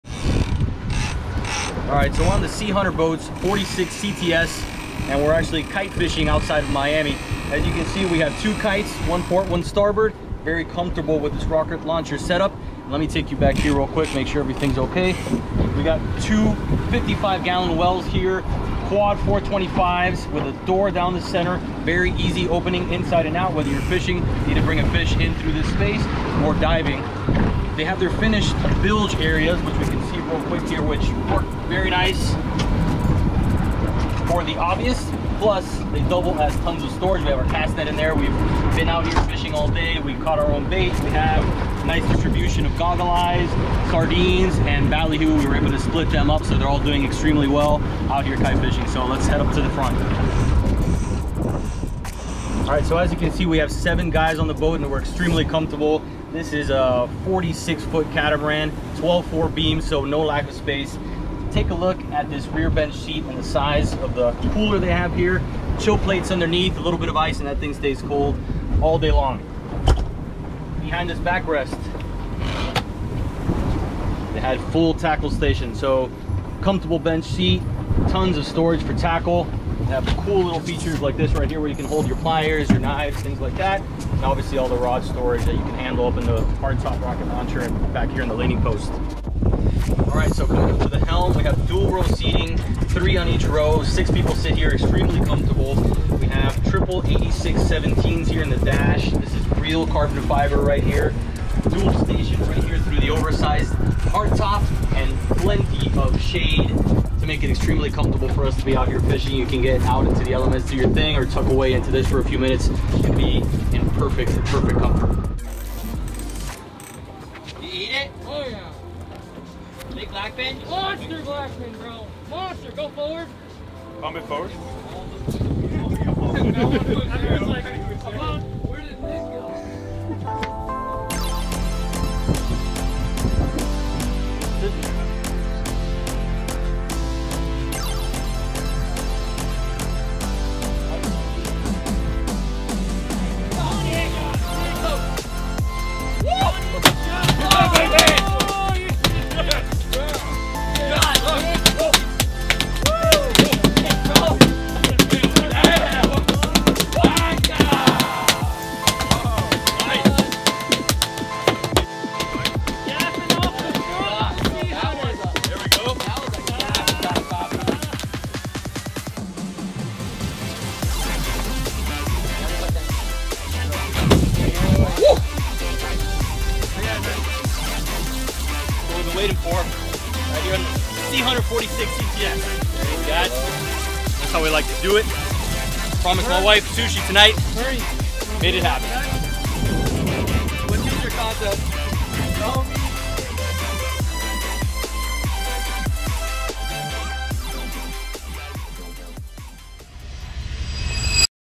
SEAHUNTER BOATS 46' CTS - Tuna Catch with a Walkthrough
We decided to feature different parts of our SeaHunter experience. We call this one "Tuna Catch with a walkthrough".

 We had an amazing opportunity to sea trial and fish the massive Sea Hunter Boats 46' CTS recently, so we made sure to shoot a ton of content.  Come along as we highlight the interior features of the boat, push it 50 mph through solid 4' seas and get some fish blood on the decks!

Gear we use to create this video:

Main Camera Canon C200
B Camera Canon EOS R
GoPro Hero 8
DJI Mavic Pro 2
Rode Wireless Mics
Small Bag
Medium Bag